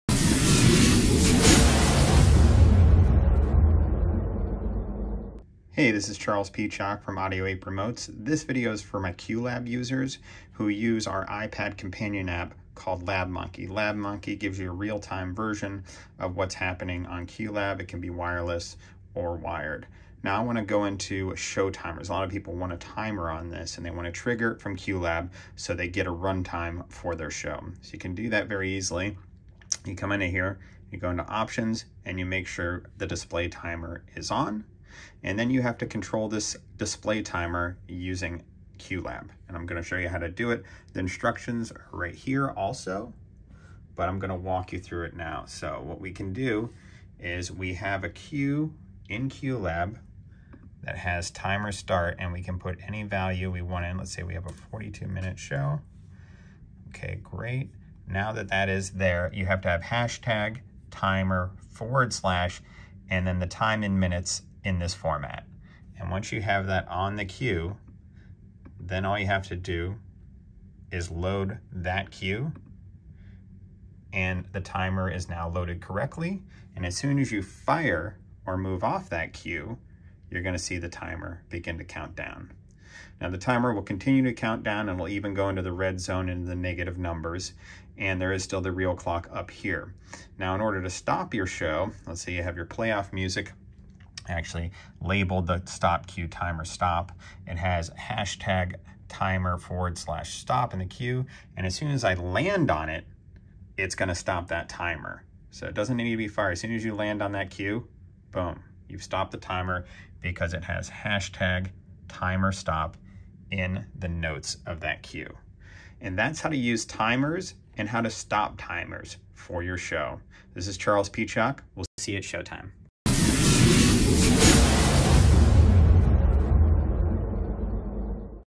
Triggers a timer on Lab Monkey app.
This video shows how to start and stop a timer on our free Lab Monkey app.

Lab Monkey is a companion app for QLab. It displays what Qlab is seeing wirelessly onto an iOS device. This allows a performer to have their laptop hidden backstage and just have an iPad onstage showing in real-time what QLab is doing.

You can buy our remote system to control it all seamlessly